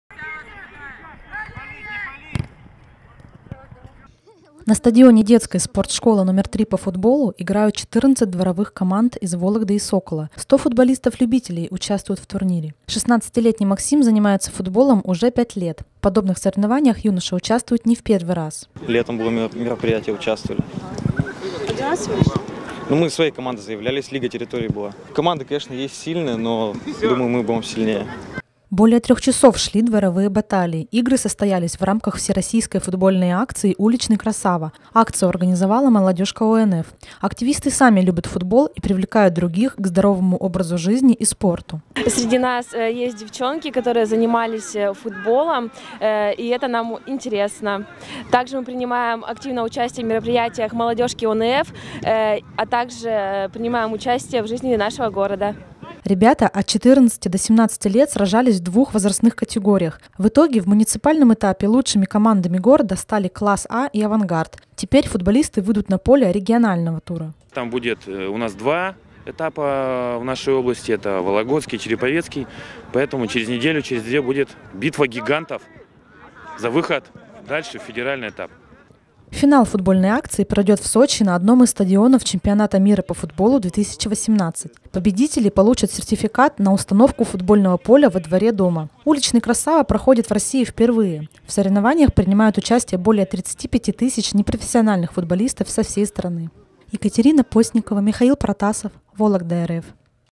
Две дворовые команды по футболу из Вологды вышли в региональный этап «Уличного красавы»
Сегодня в областной столице на стадионе СДЮСШОР № 3 по футболу прошли игры муниципального этапа Всероссийской акции «Уличный красава». Жеребьевка состоялась 8 ноября в региональном отделении ОНФ.